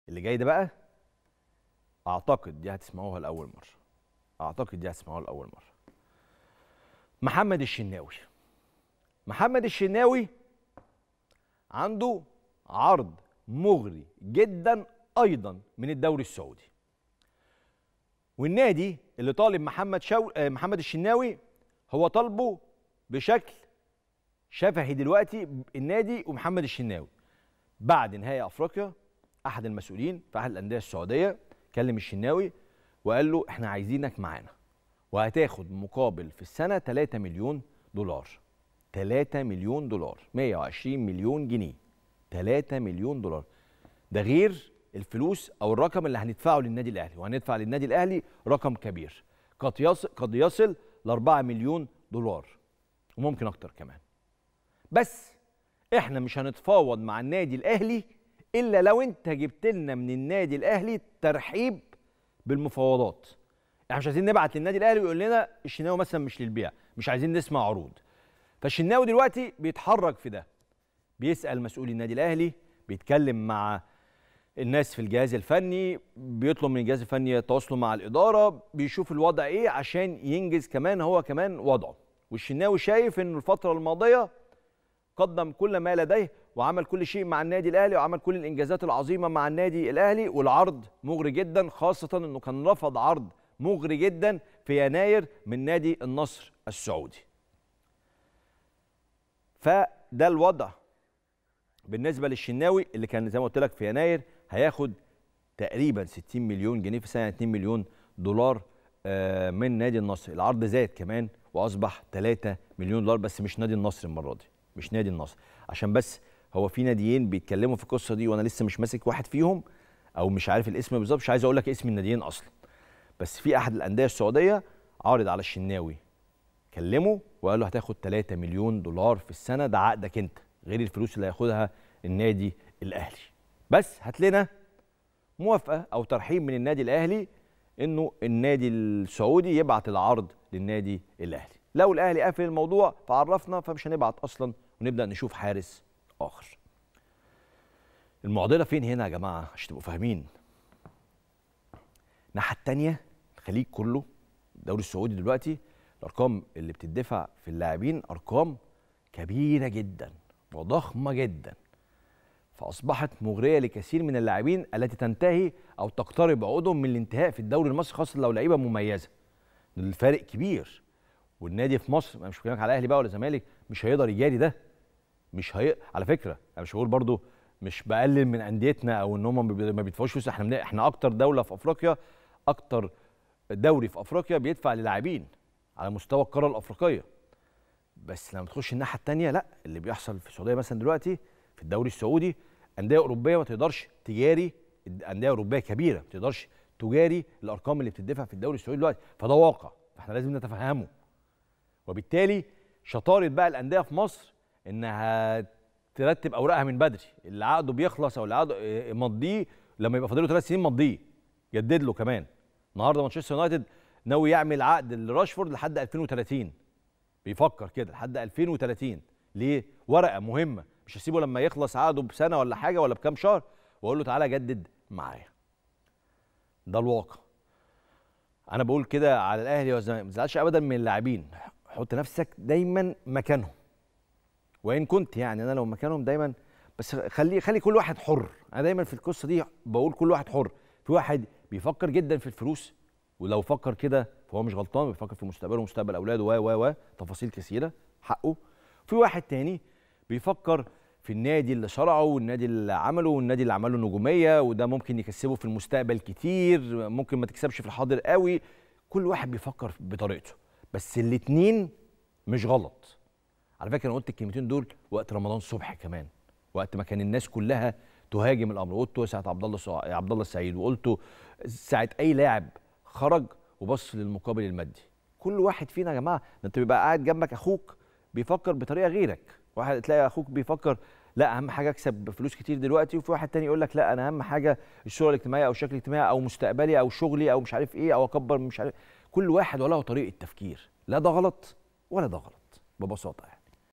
"عرض سعودي بـ 120 مليون جنية في الموسم لـ محمد الشناوي"😱😱كريم رمزي يكشف كواليس هتصدمك 🔴🦅👇🏻
"عرض سعودي بـ 120 مليون جنية في الموسم لـ محمد الشناوي"😱😱كريم رمزي يكشف كواليس هتصدمك عن العرض المغري لكابتن النادي الأهلي 🔴🦅👇🏻